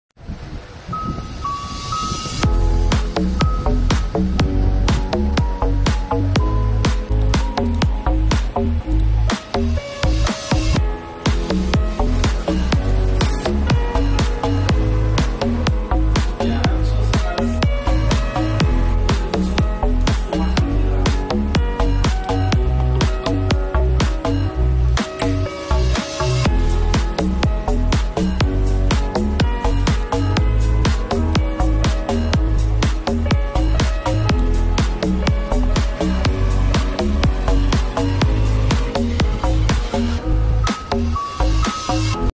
Laser marking machine with camera
The marking machine has its own camera function, and the marking content can be clearly seen through the computer

Best Ragards
Amy

Phone number/Whats app/ Wechat number:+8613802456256